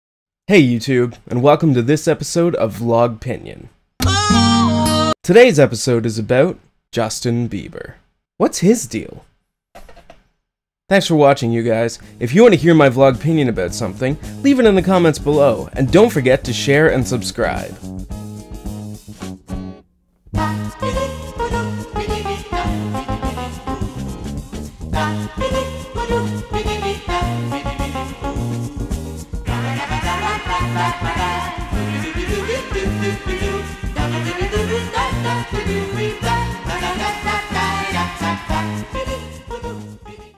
Adam's Vlogpinion Episode 1: Justin Bieber
Adam tells it like it is about today's topic: Justin Bieber.

You can see a new Vlogpinion every Monday!

Want to know Adam's Vlogpinion about something? Post a comment about it below!